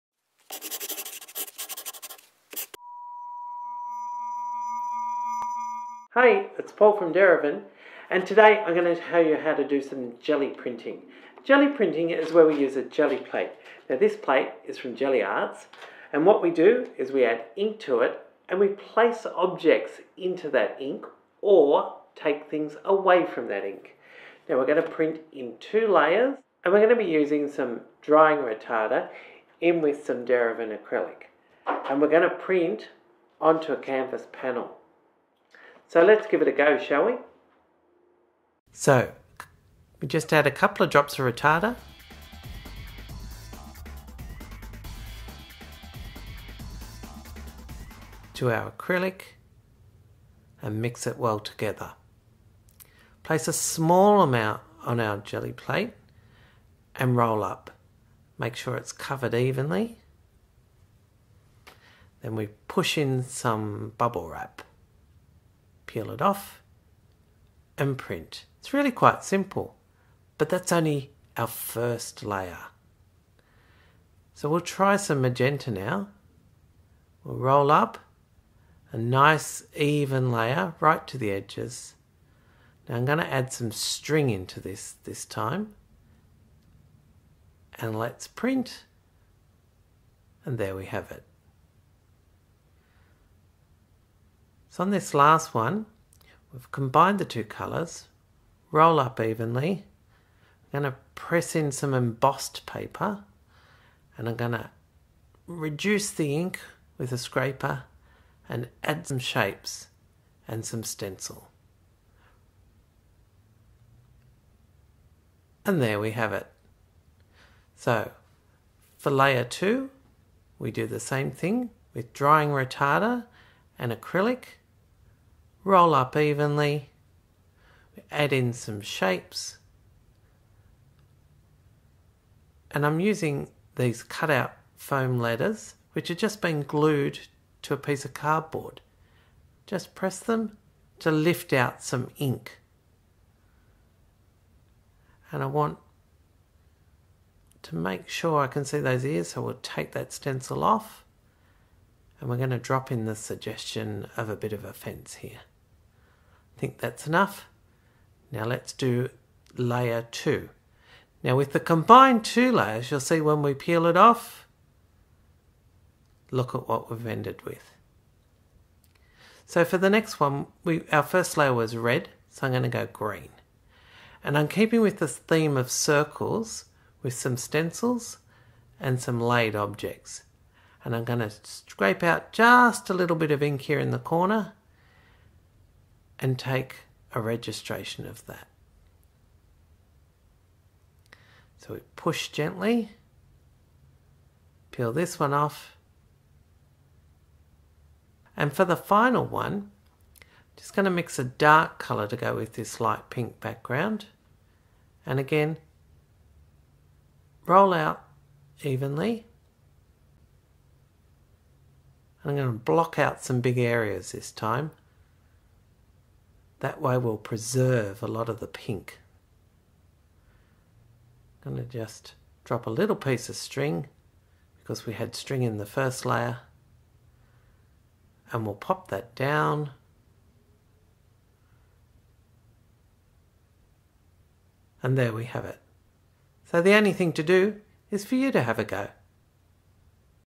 Demonstration: Gelli Printing on Canvas Boards – Derivan Acrylics
In this video @ArtistPaulMcCarthy  demonstrates Gelli plate monotype printing onto canvas boards   -using  stencils, working with textured and natural materials, multiple-stage printing.